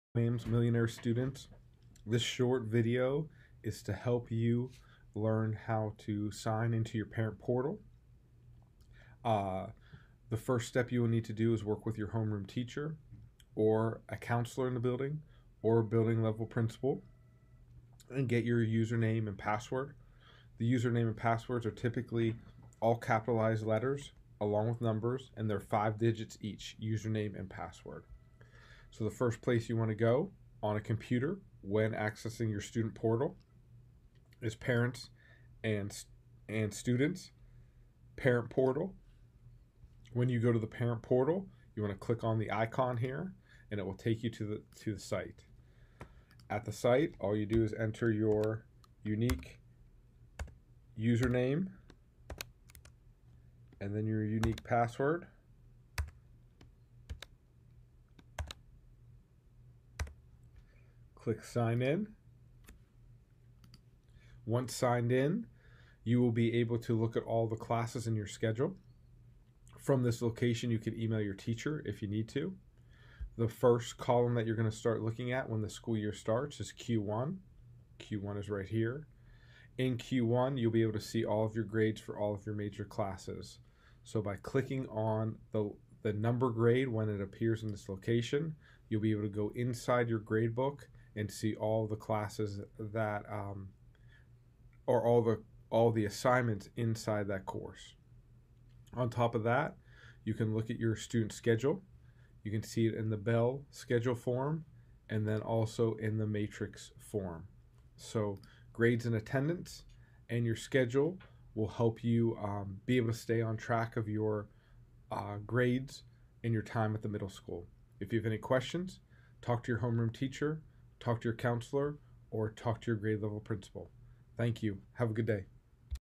How to Access Student PowerSchool
Watch this video to learn how to find and access Student PowerSchool